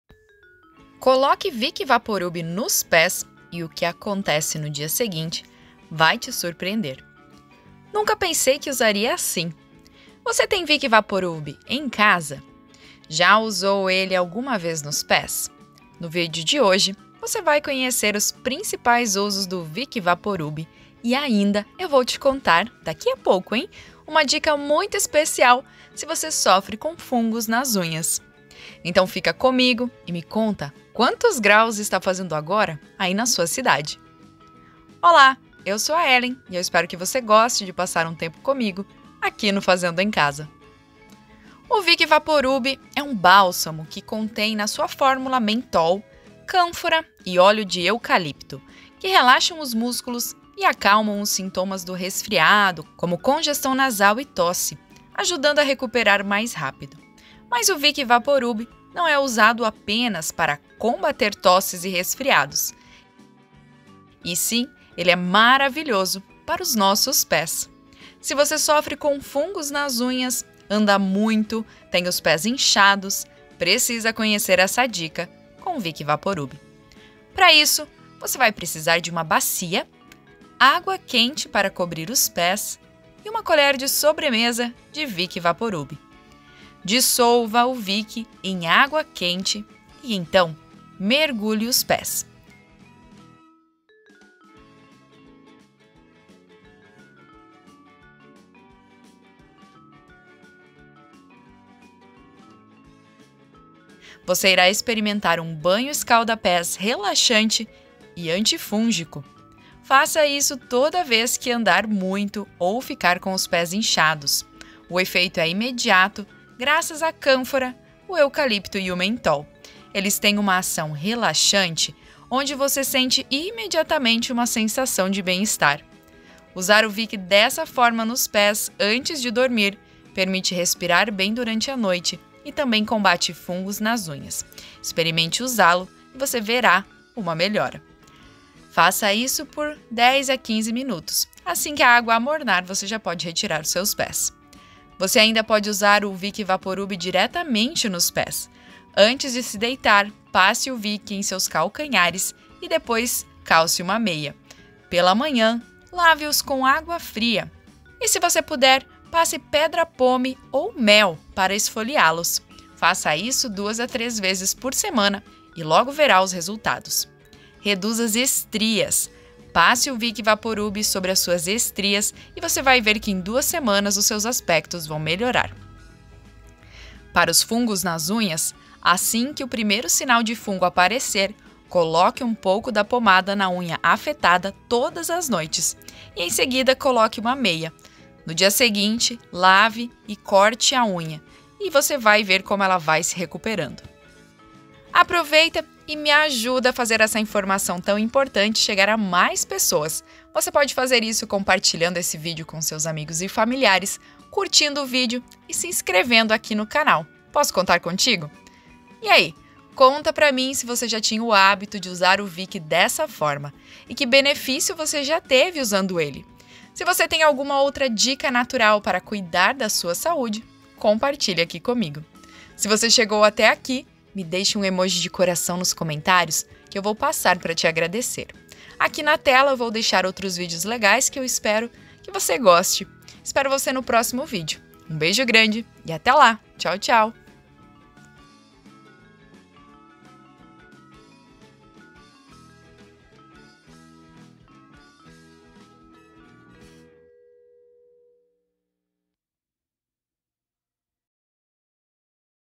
COLOQUE VICK VAPORUB NOS PÉS E O QUE ACONTECE NO DIA SEGUINTE VAI TE DEIXAR CHOCADO 😱
No vídeo de hoje você vai conhecer os principais usos do Vickvaporub e eu ainda vou te contar daqui a pouco uma dica muito especial se você sofre com fungos nas unhas.

O Vicks Vaporub é um bálsamo que contém na sua fórmula mentol, cânfora e óleo de eucalipto que relaxam os músculos e acalmam os sintomas de resfriado, como congestão nasal e tosse, ajudando a recuperar mais rápido. Mas o Vick Vapo Rub não é usado apenas para combater tosses e resfriados, mas é maravilhoso para os nossos pés.

Se você sofre com fungos nas unhas, anda muito, tem os pés inchados, precisa conhecer essa dica com vick vaporub.